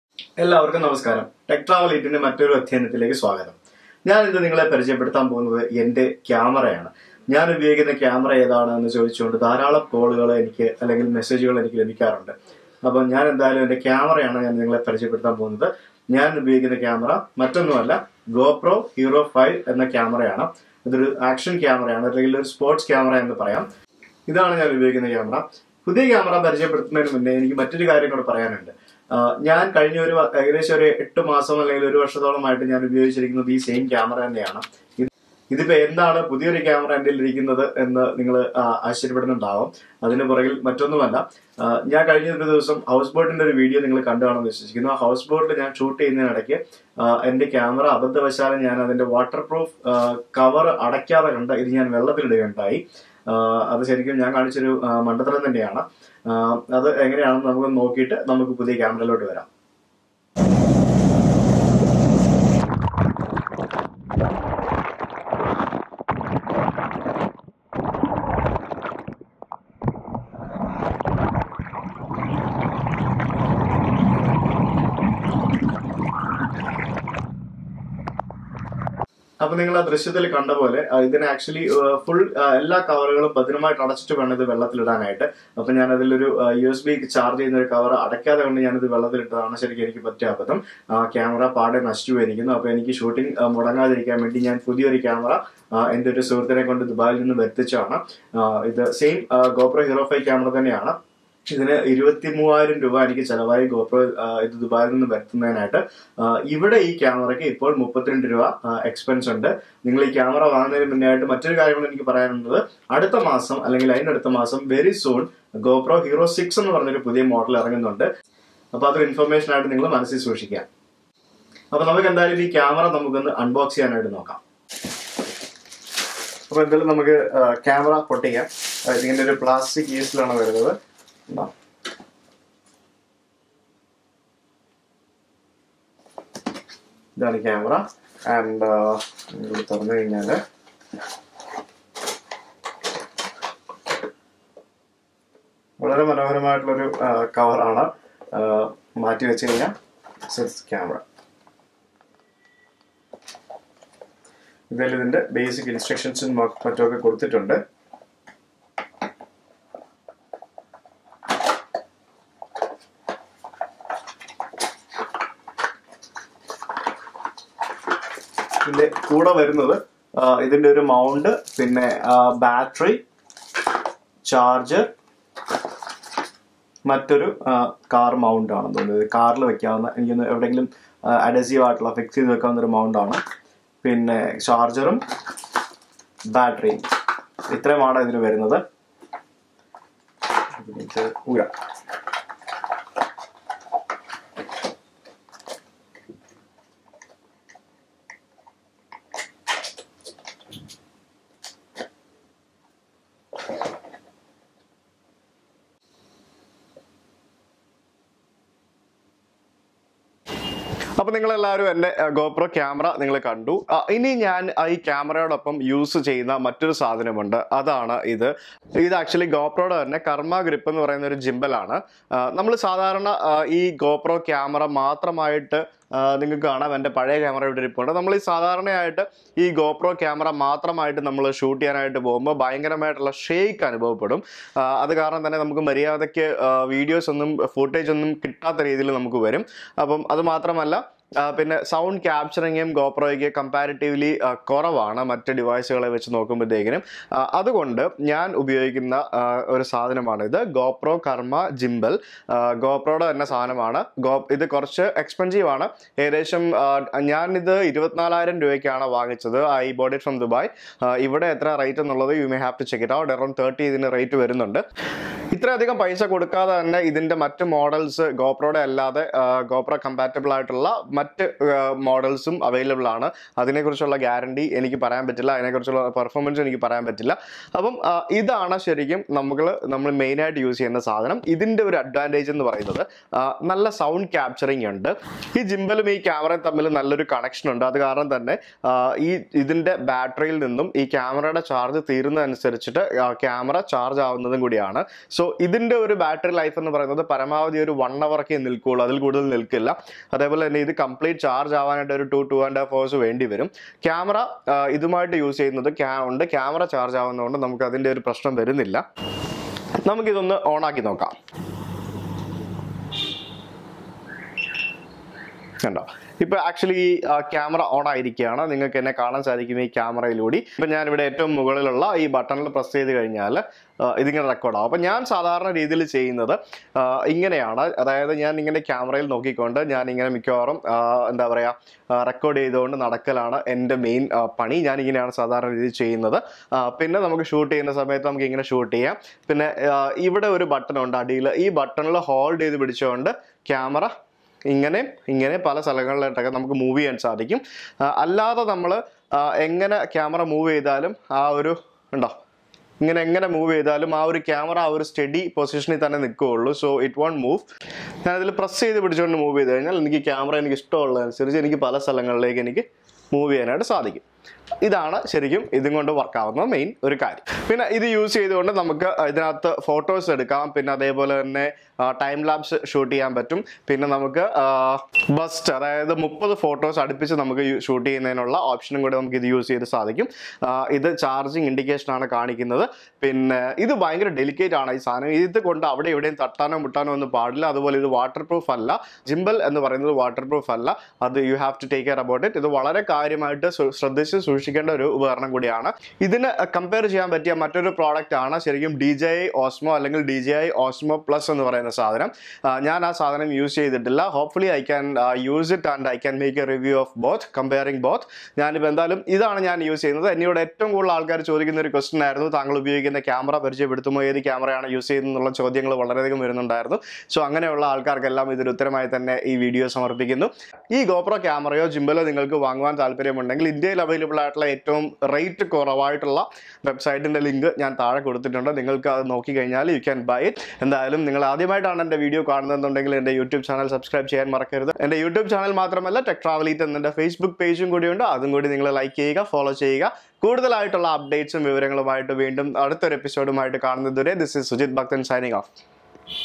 GoPro Hero 5 Camera & Karma Grip for Vlogging, Review by Malayalam Technology & Travel Blogger
GoPro Hero 5 Camera & Karma Grip Gimbal Malayalam Review by Malayalam Technology & Travel Blogger, Sujith Bhakthan. ഞാൻ ഉപയോഗിക്കുന്ന ക്യാമറ - ഗോപ്രോ ഹീറോ 5 & കർമ്മാ ഗ്രിപ്പ് സ്റ്റെബിലൈസർ. കൂടുതൽ വിവരങ്ങൾക്ക്